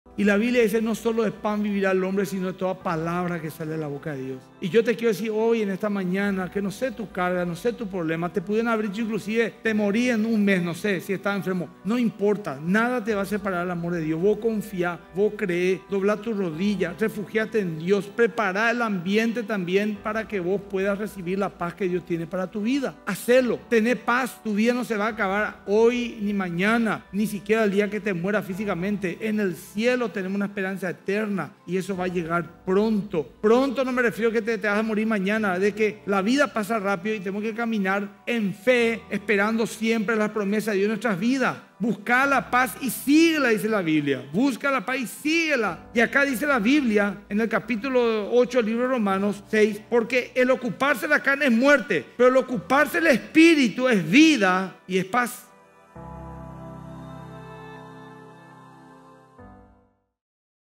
Esperanza eterna - Pr. Emilio Agüero Esgaib
MDI431 | Quienes hemos rendido nuestra vida a Cristo, tenemos la certeza de que nuestra vida no acabará ni siquiera el día que partamos de esta tierra. ¡Nosotros tenemos una esperanza eterna! Esta verdad nos debe animar a caminar con fe cada día.

➡️ Fragmento de la prédica Serie de Romanos 8 - Parte 1: "La seguridad de salvación", que puedes verlo